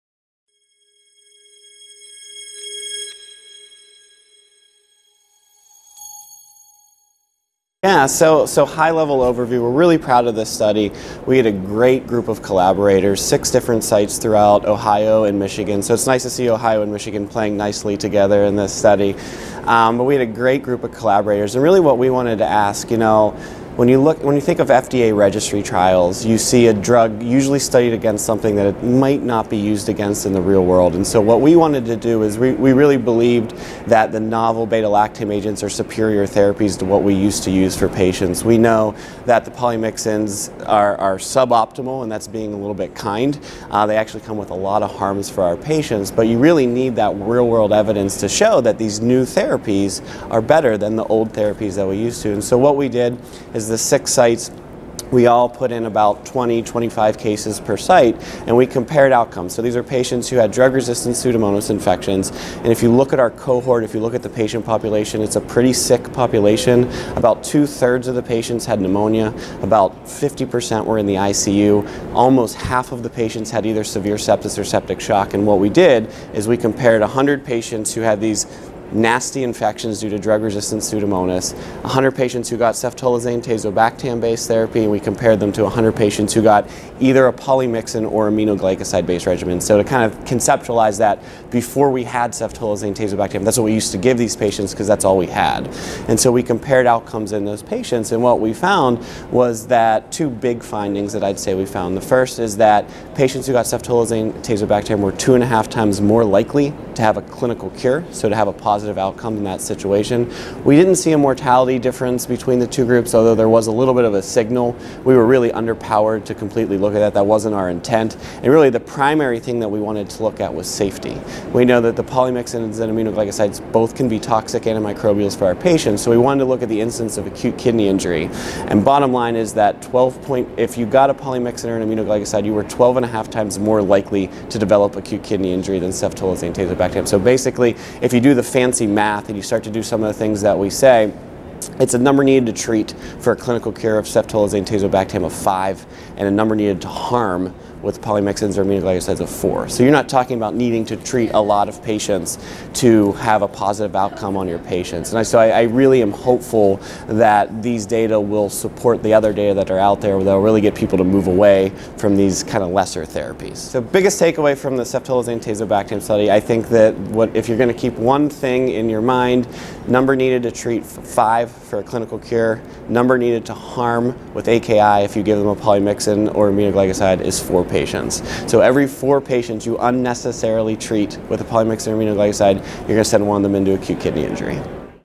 Jason Pogue, PharmD: Ceftolozane/Tazobactam for Drug-Resistant P aeruginosa
Jason Pogue, PharmD, discusses his research evaluating ceftolozane/tazobactam for drug-resistant Pseudomonas aeruginosa.